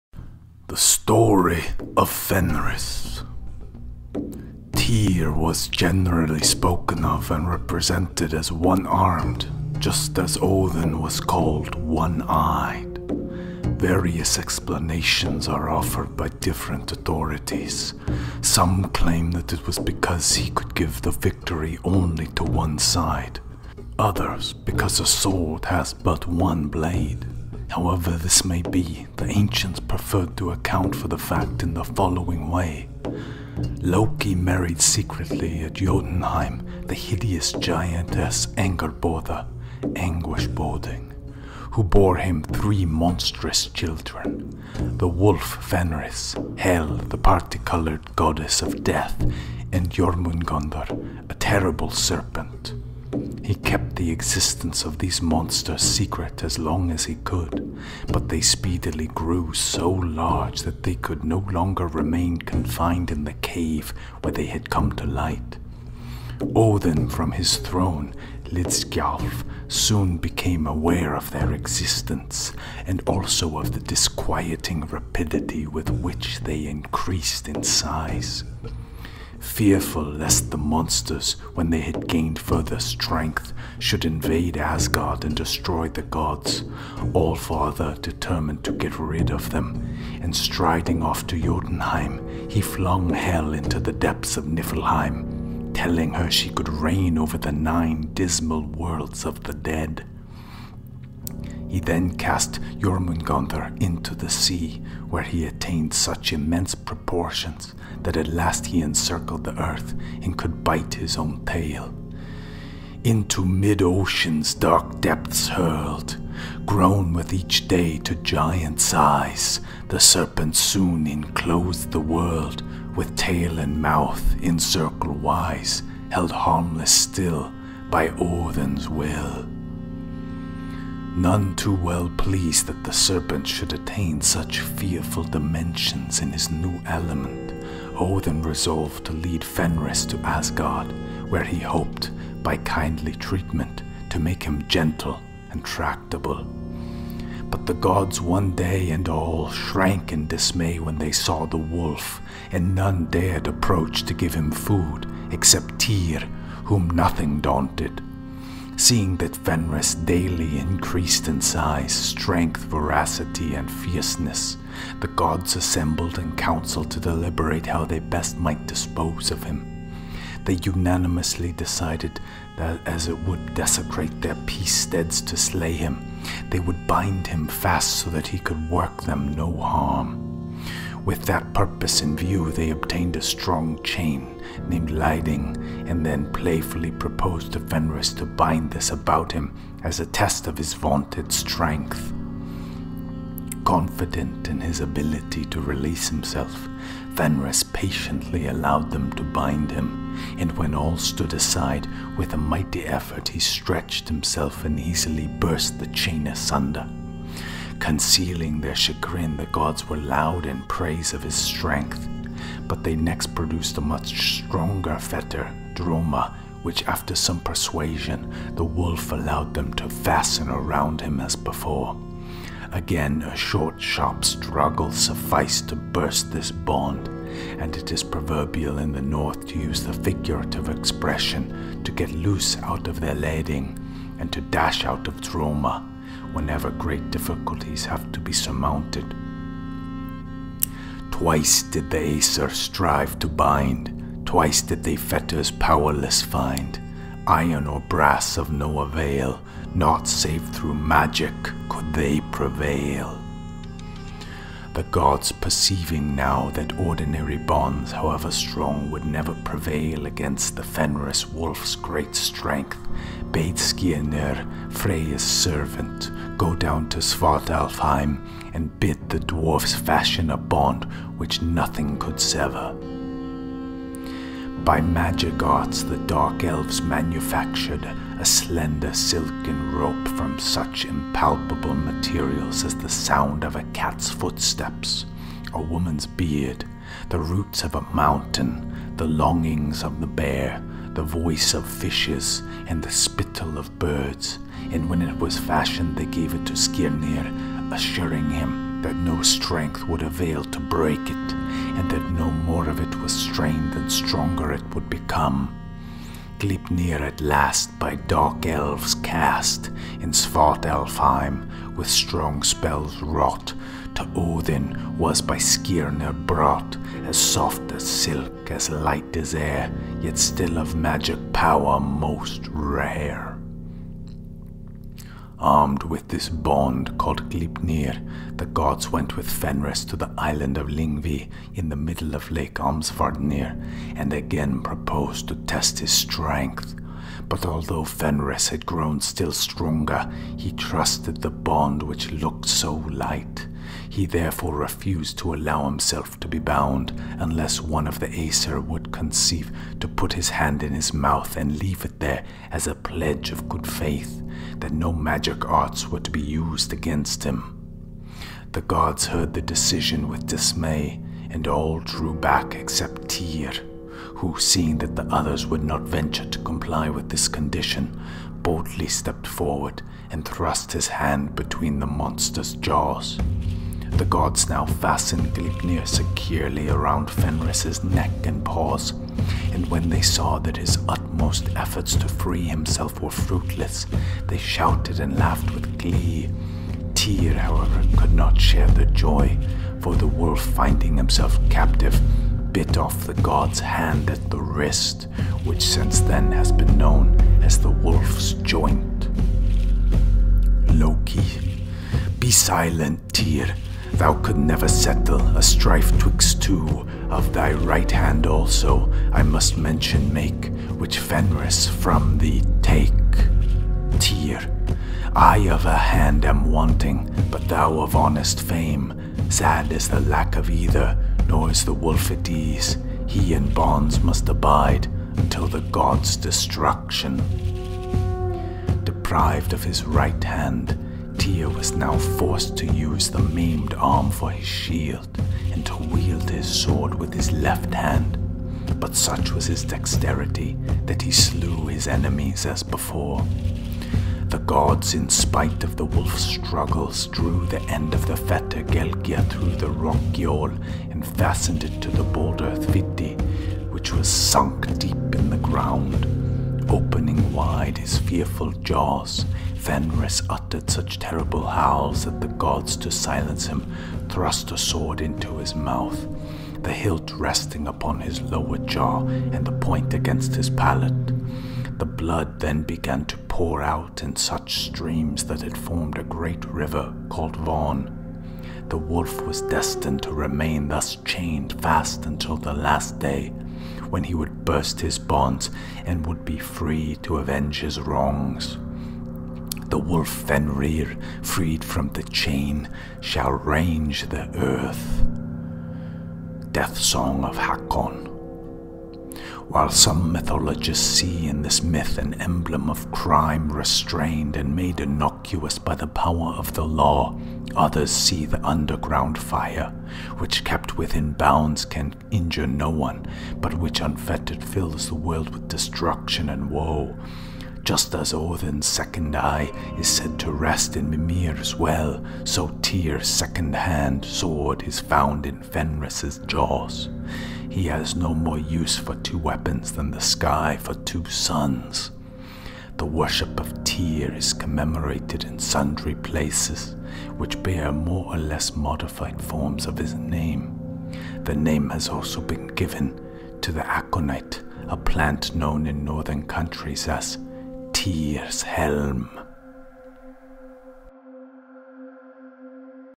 LOKI AUDIOBOOK 🗡️ Animated 🗡️ Fenris the Wolf of NORSE Mythology
A reading of the Story of Fenris (Fenrir) the powerful wolf of Norse mythology and son of Loki. From the public domain book Myths of the Norsemen from the Eddas and Sagas by H.A. Guerber (1909) from Project Gutenberg, in the public domain.

Animation, film, music and narration by myself, Ulf Crow.